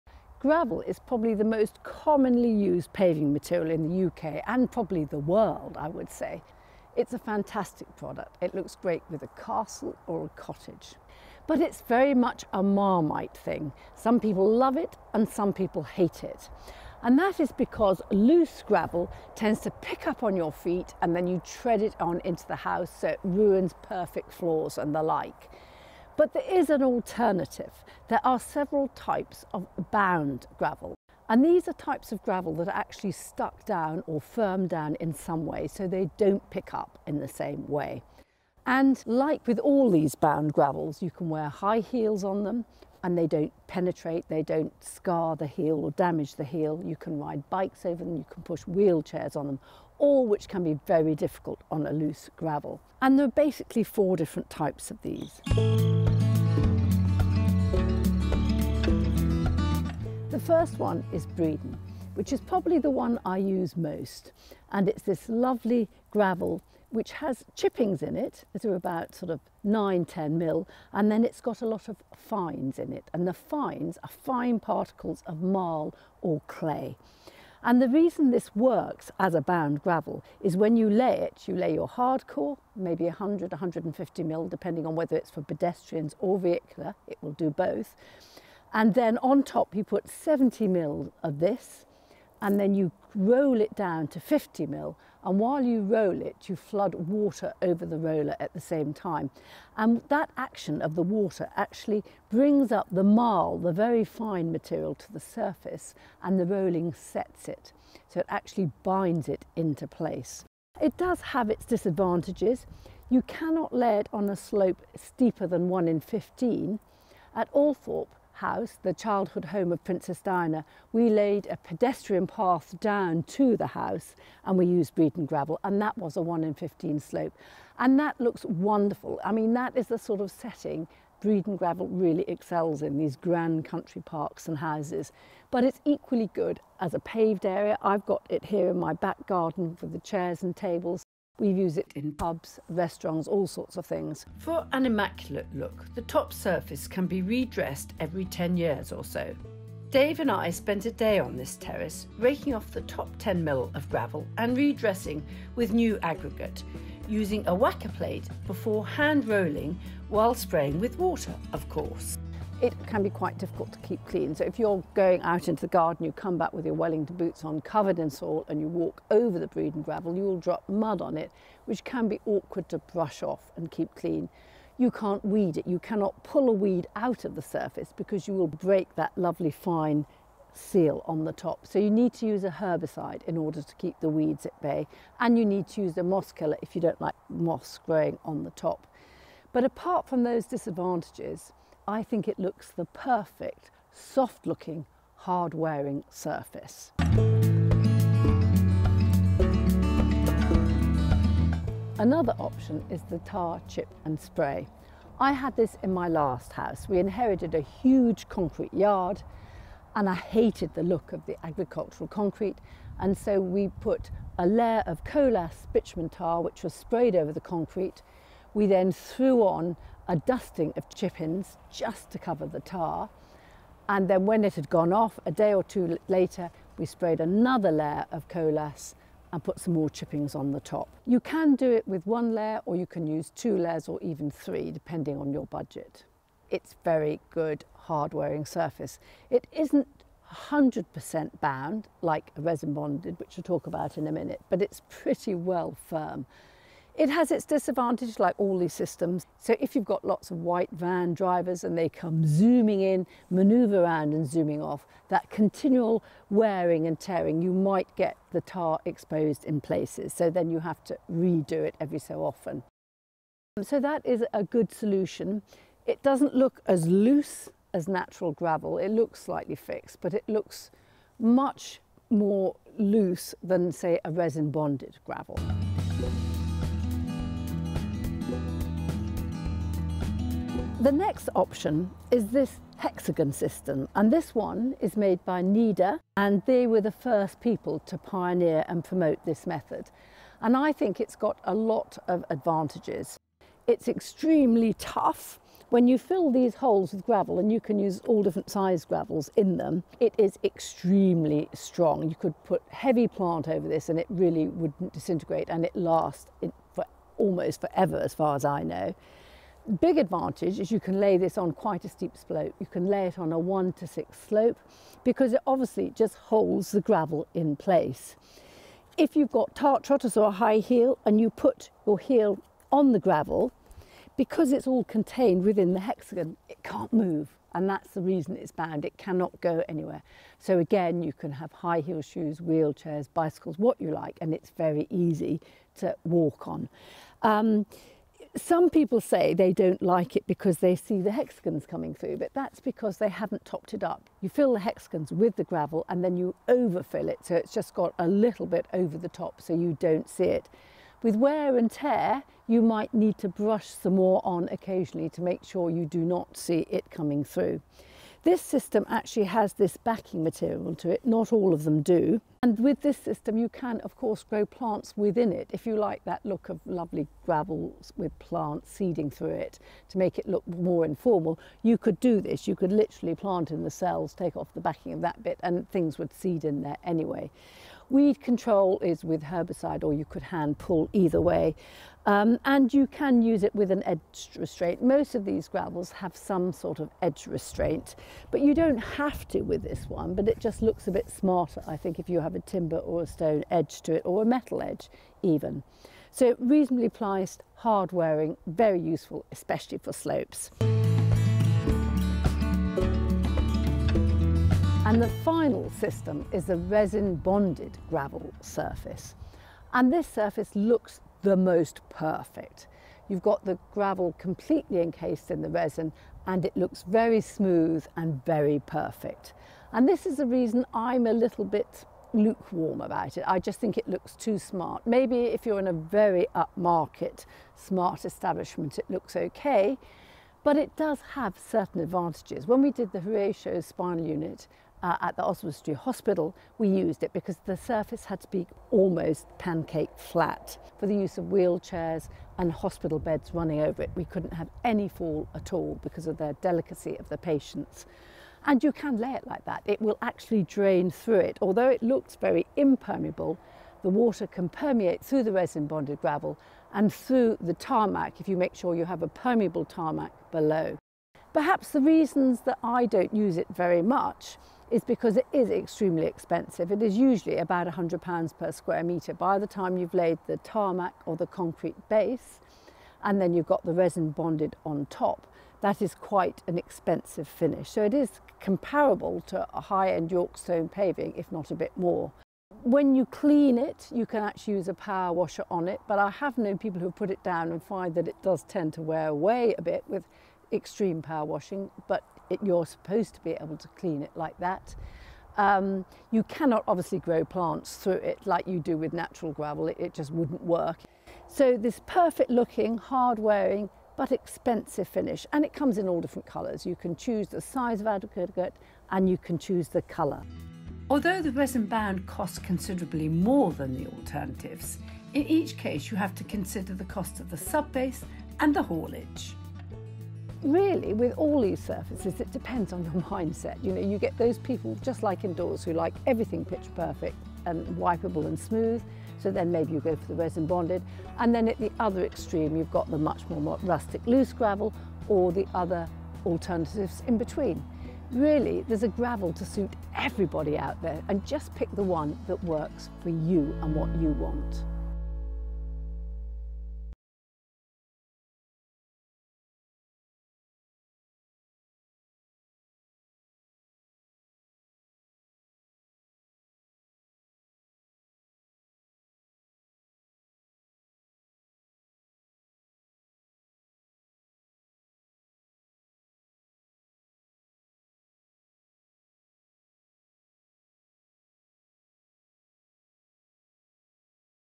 BOUND Gravel - ALL the Alternatives
Bunny's Guide to the different types of BOUND Gravel - a great alternative to the Loose Stuff.